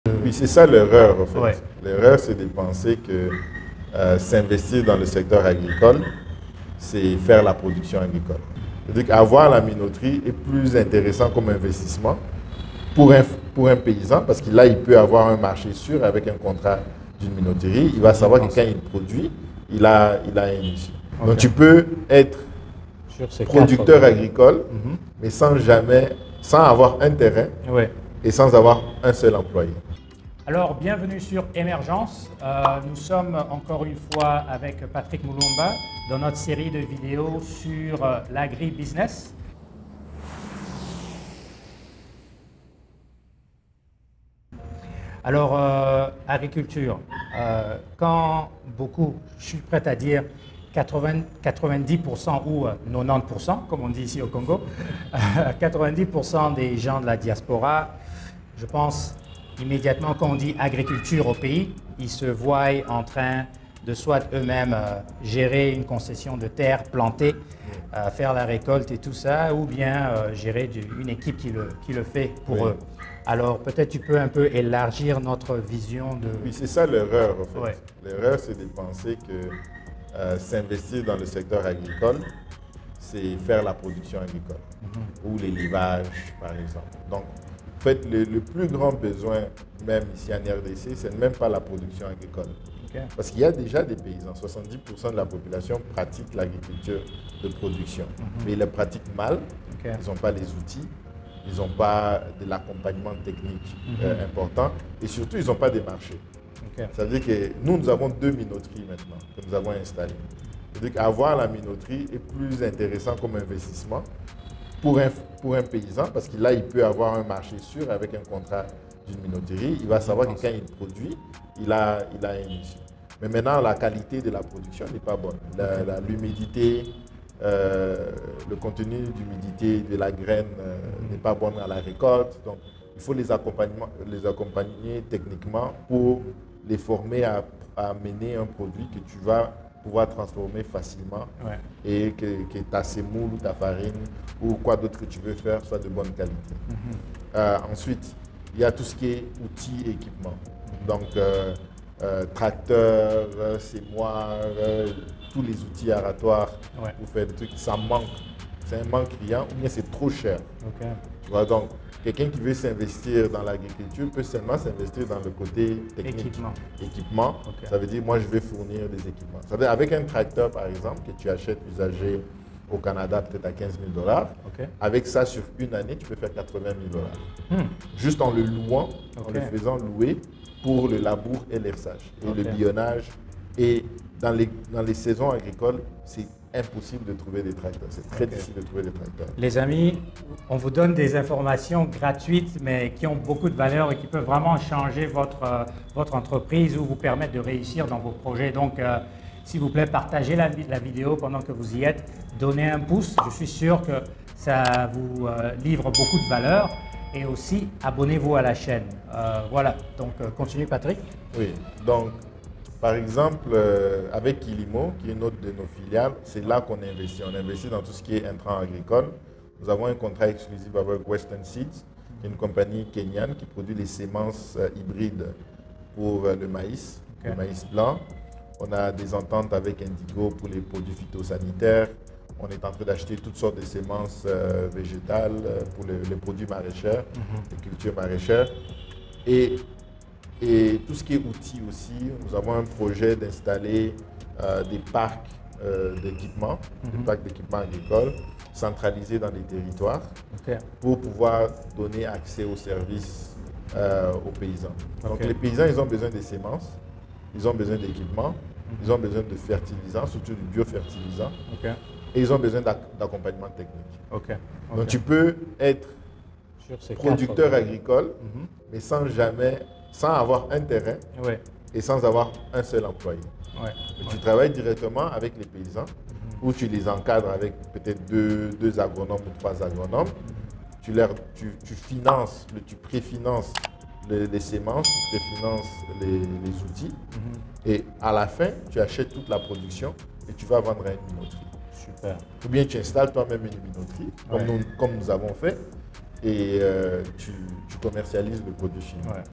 Comment devenir MILLIONNAIRE dans l'agrobusiness?
M. Patrick Mulumba, Président du Groupe Logix Africa, nous présente les créneaux d'affaires les plus rentables dans le domaine de l'agrobusiness. Il y a bien plus que la simple production agricole.

Les futurs millionnaires saisiront ces opportunités immédiatement!

Rendez-vous au sommet 😉

Suivez ici l'intégralité de notre entretien avec M. Mulumba sur les "Opportunités en or de l'agrobusiness en Afrique" :